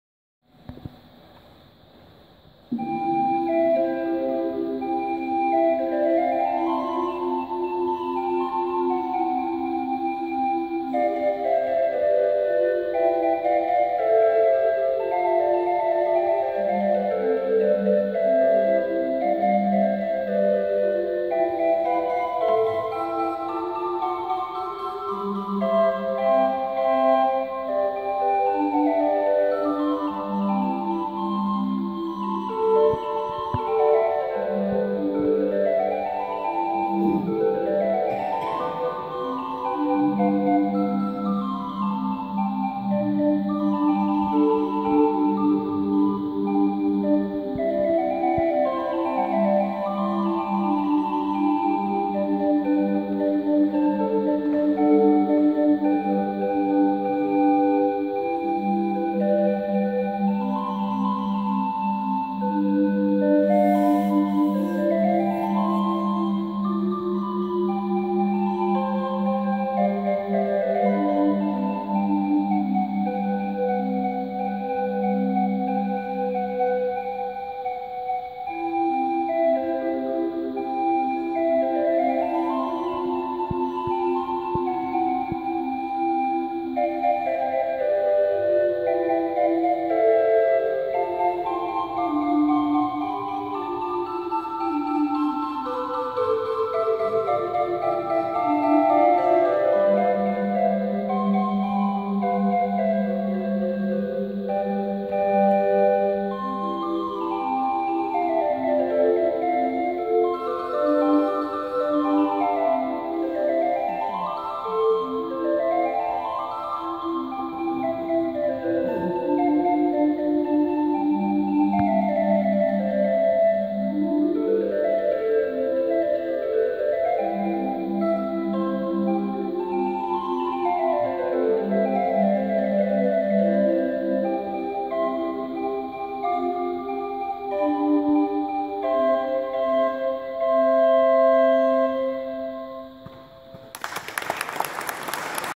meine kleine 20er Jäger & Brommer Drehorgel von 2009 klingt in der Kirche
meine kleine 20 er Jäger & Brommer Drehorgel von 2009 klingt in der Kirche